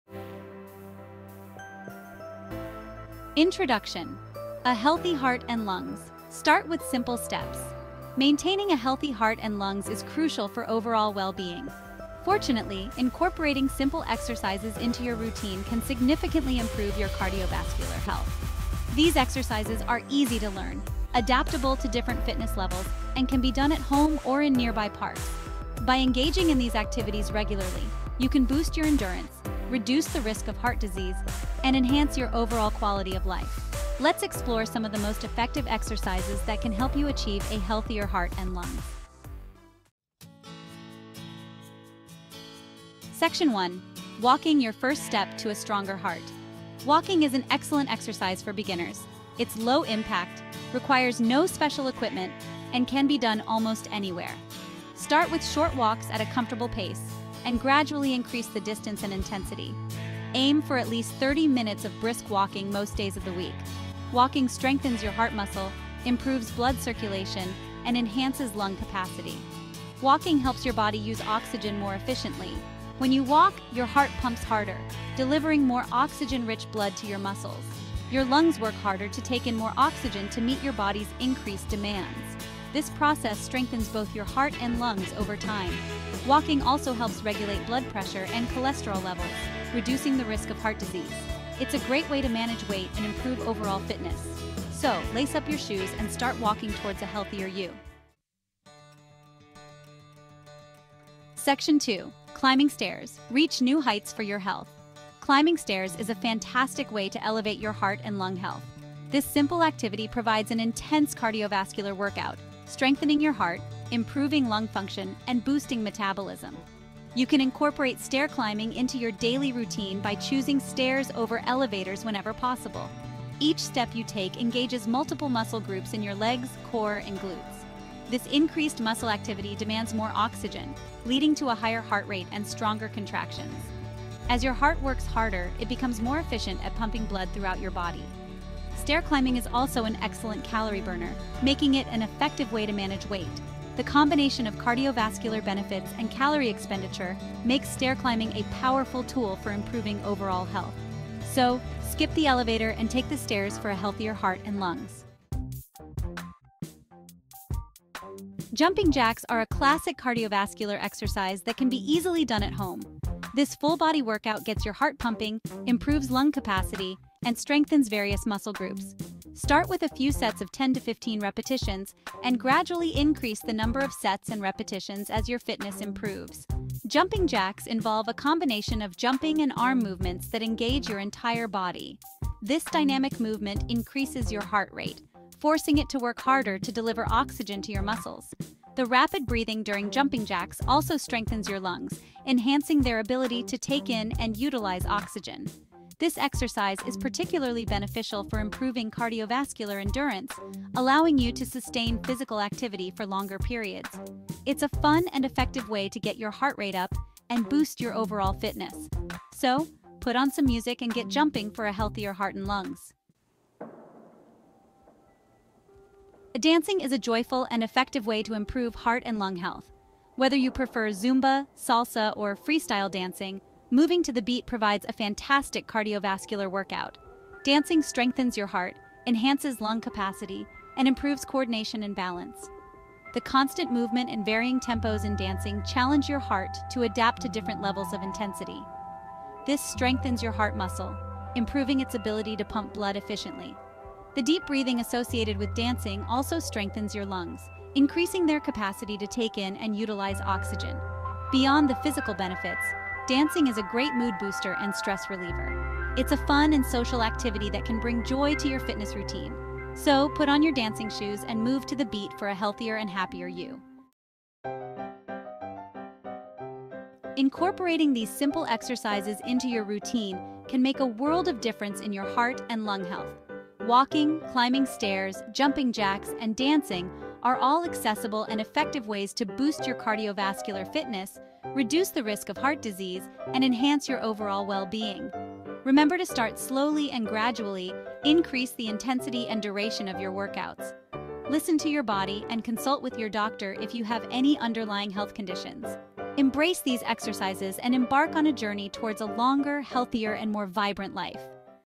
Simple Exercises to improve Heart & Lung Functioning
In this informative video, we delve into the remarkable benefits of exercise on heart and lung health. Discover how regular physical activity can significantly improve the functioning of these vital organs, leading to a healthier and more resilient cardiovascular system. From boosting lung capacity to strengthening the heart muscle, exercise offers a multitude of advantages that extend far beyond just physical fitness. Join us as we explore the science behind these benefits and learn practical tips for incorporating exercise into your daily routine to promote optimal heart and lung function. Don't miss out on the opportunity to transform your health and vitality through the power of exercise!